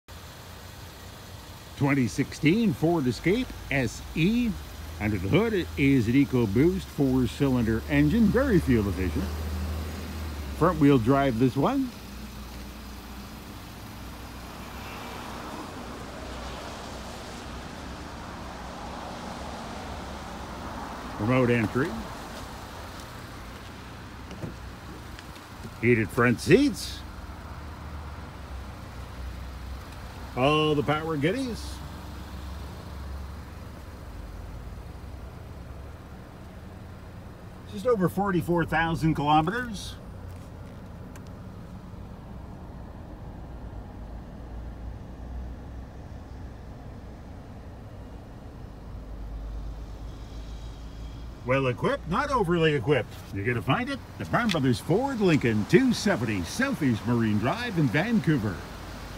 2016 Ford Escape SE FWD. Stk: OP19488
Local, SE, FWD, low km's, 240 HP 2.0L 4 cyl, 6 spd auto, remote entry, backup camera, Sync, fog lamps, tonneau cover, roof rack, aluminum wheels and more.